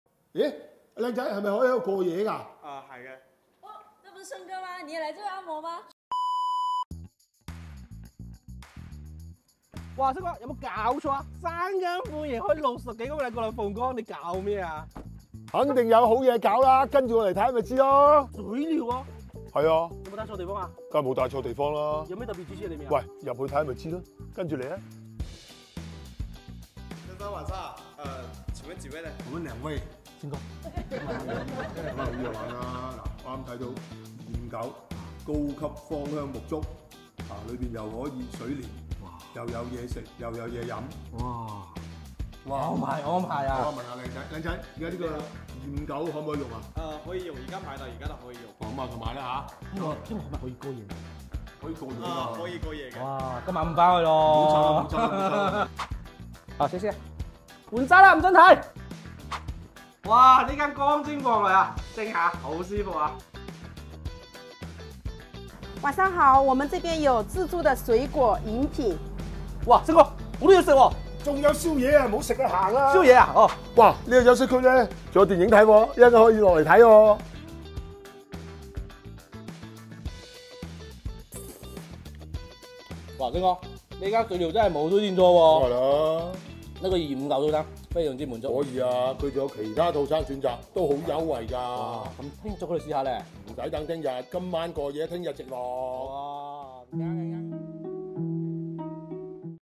三更半夜开了六十多公里来凤岗 就为了体验一下这家水疗会所 果然有特别之处 还能过夜 爽 A Great Spa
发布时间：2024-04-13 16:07